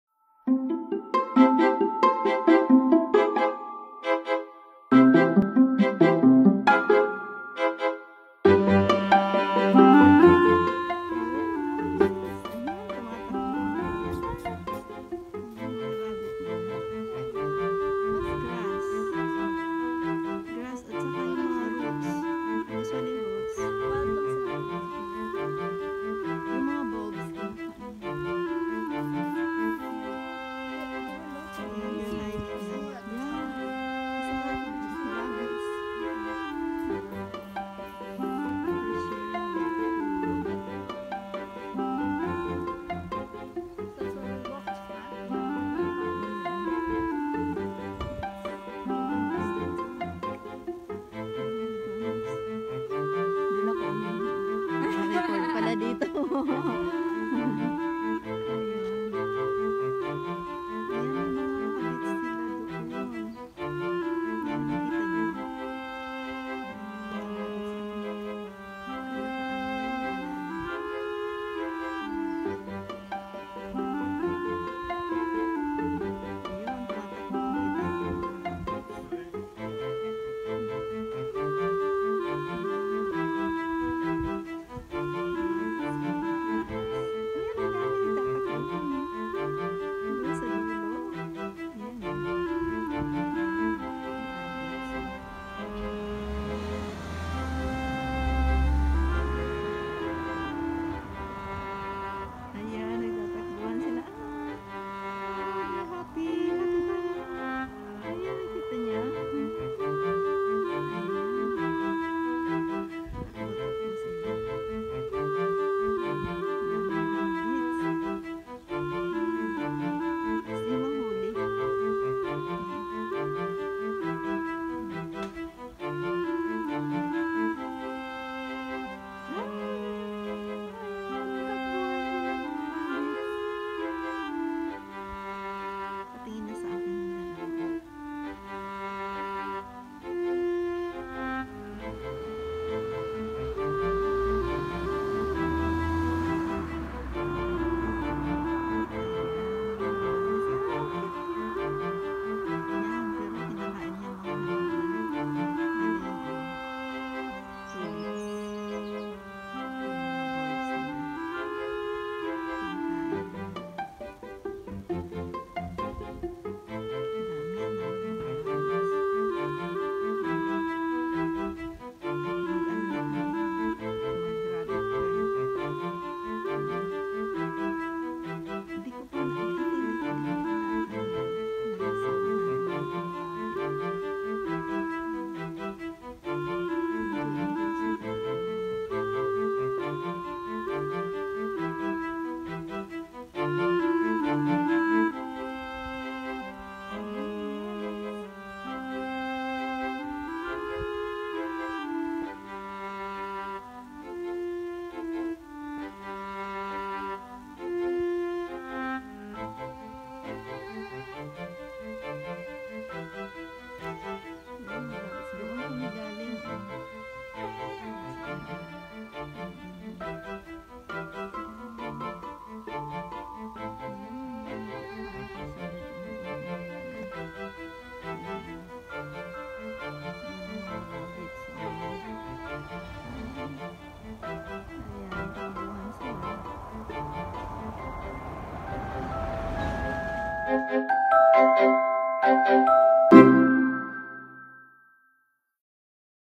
Flock of wild Rabbits
These wild rabbits are playing freely along with the Sheeps